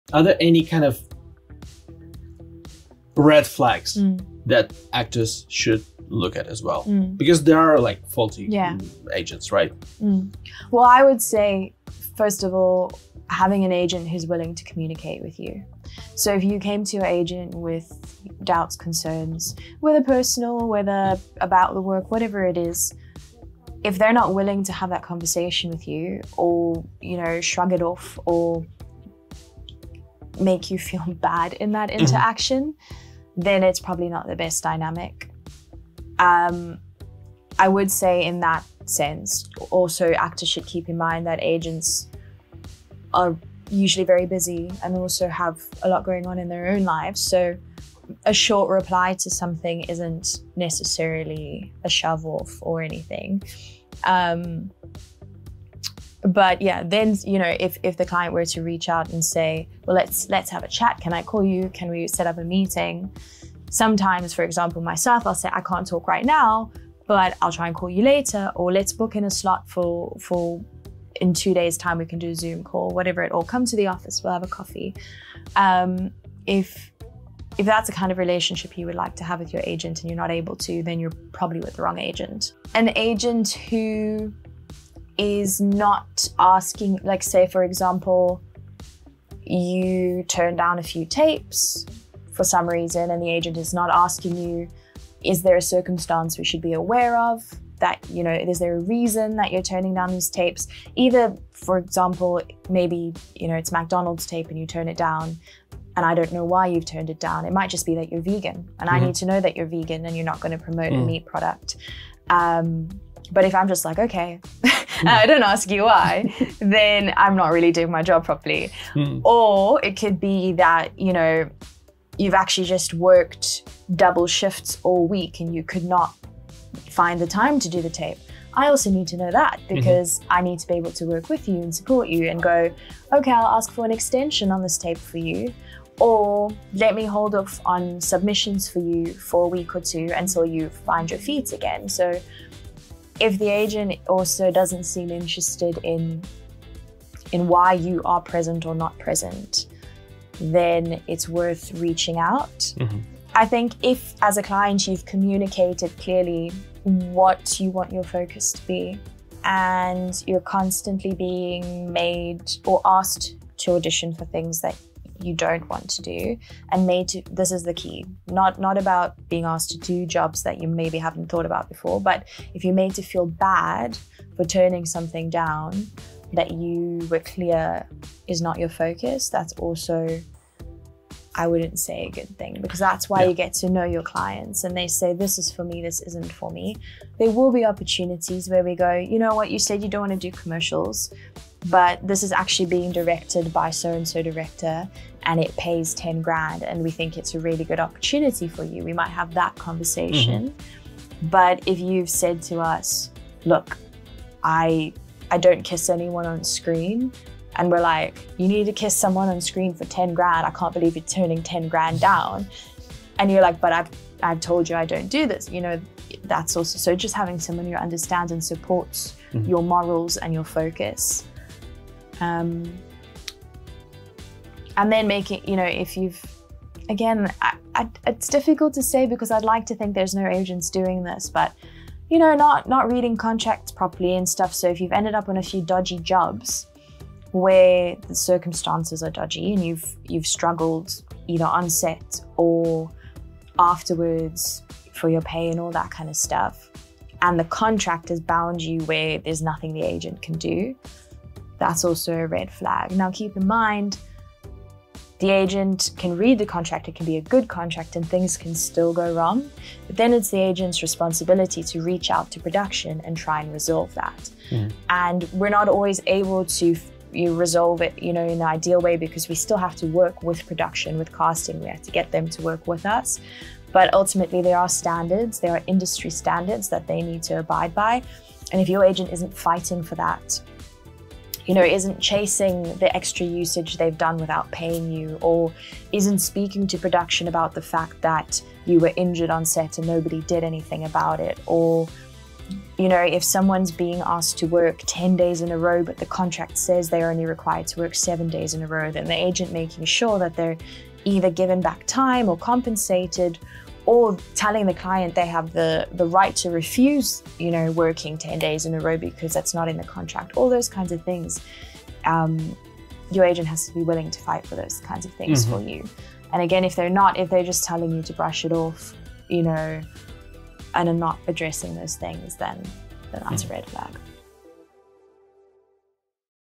How to recognize a BAD ACTING AGENT?
Acting agent Nicole Ricardo on how to define bad acting agent... or an agent that's not for you.

Beyond Reel Talk is a podcast where I invite entertainment industry professionals to ask them about their journey, their professions, challenges and motivation.